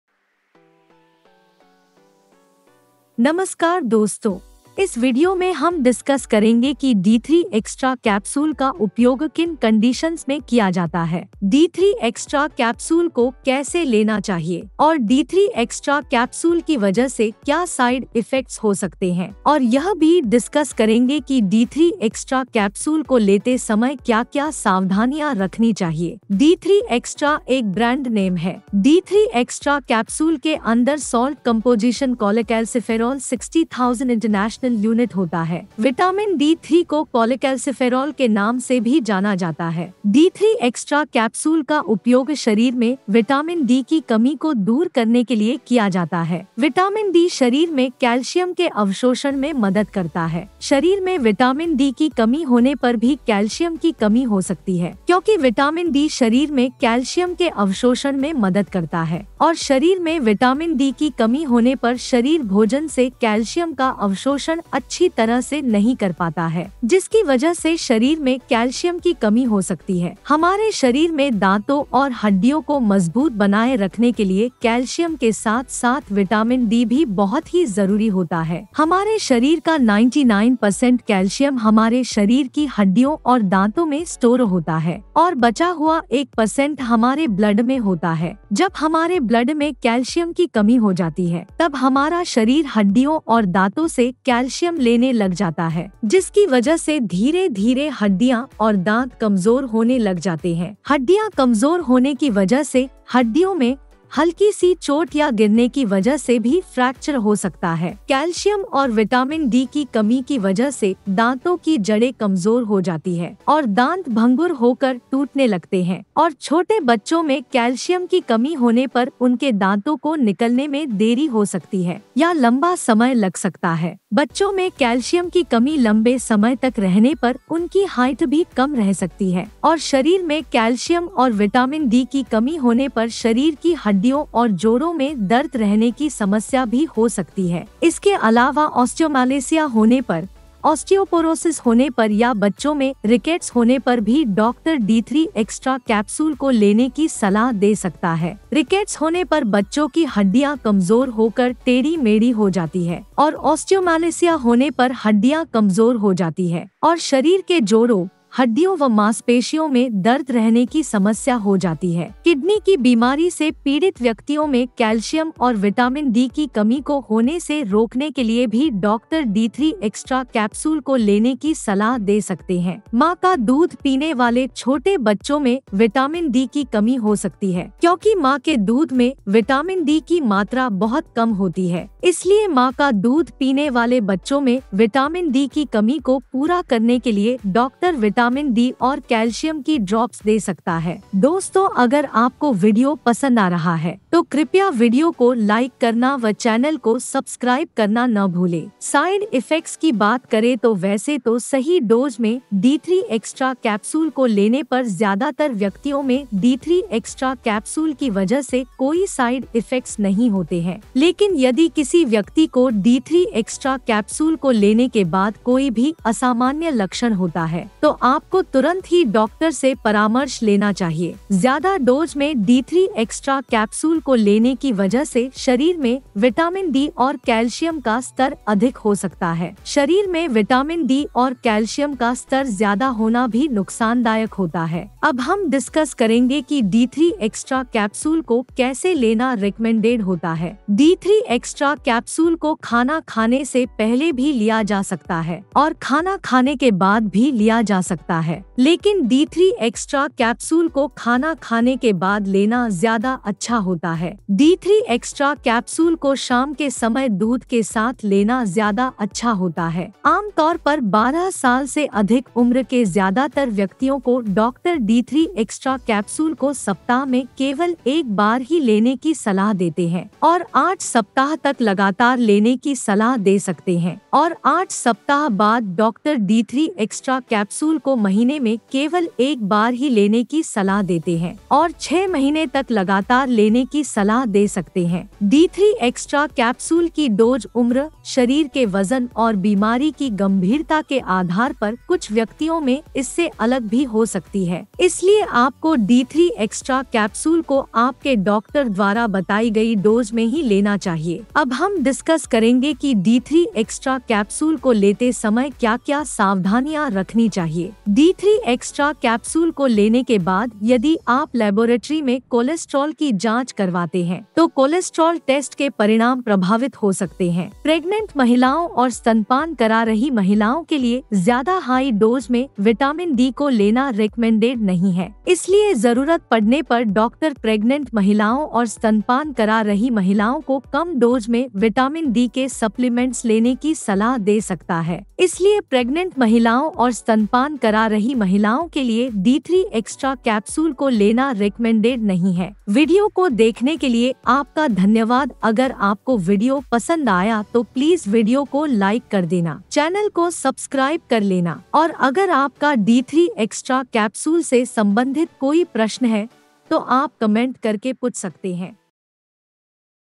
D3 Extra Capsules Uses & Side Effects in Hindi, D3 Extra Capsules Ke Fayde Aur Nuksan
d3 extra capsules uses & side effects in hindi | d3 extra capsules ke fayde aur nuksan | how to use d3 extra capsules. in this video, we will discuss what are the uses and benefits of d3 extra capsules. How to use d3 extra capsules. We will also discuss what are the side effects of d3 extra capsules. Watch the complete video for detailed review of d3 extra capsules in hindi.